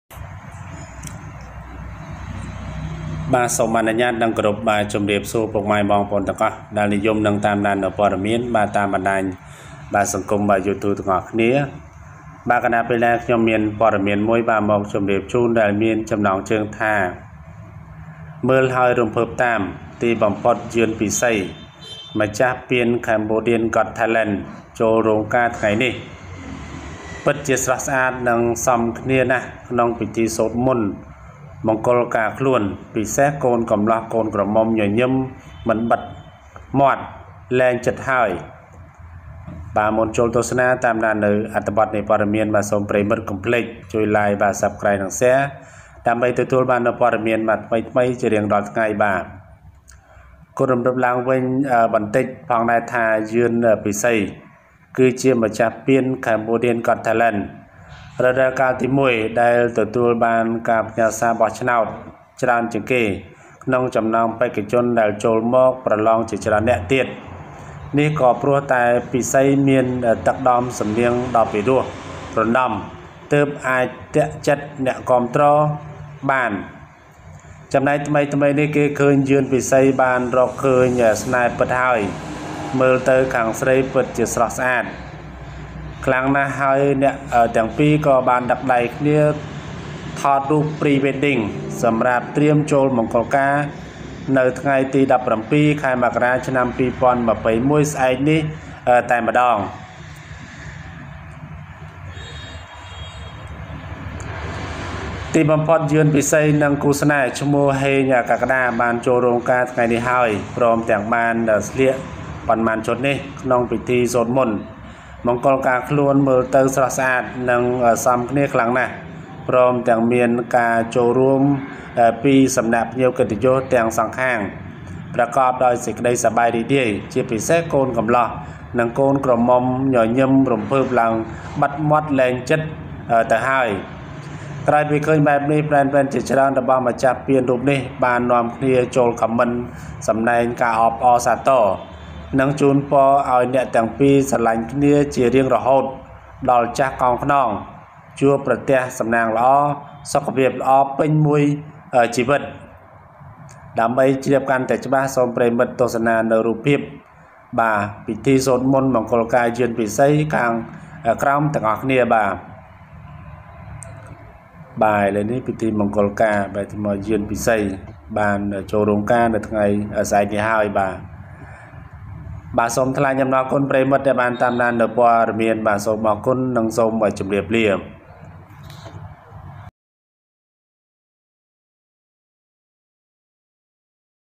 មើលហើយ រំភើបតាម ! ទីបំផុត យឿន ពិសី ​ម្ចាស់​ពាន Cambodia’s Got Talent ចូលរោងការថ្ងៃនេះ បាត់, ពិតជា
Watch and be excited!  Finally, Yoeun Pisey, the winner of Cambodia’s Got Talent award, went to the wedding today, really beautiful and fit in the wedding blessing ceremony, especially the bride and groom are no longer smiling (video)